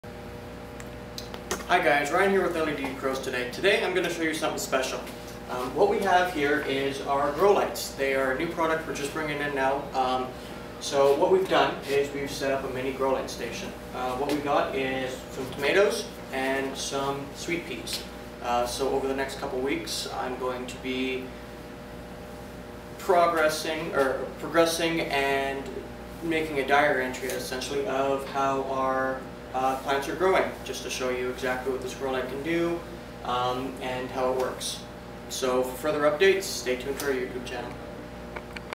LEDPros LED Grow Light VLOG
Just the start of a record for our plants and grow light experiment.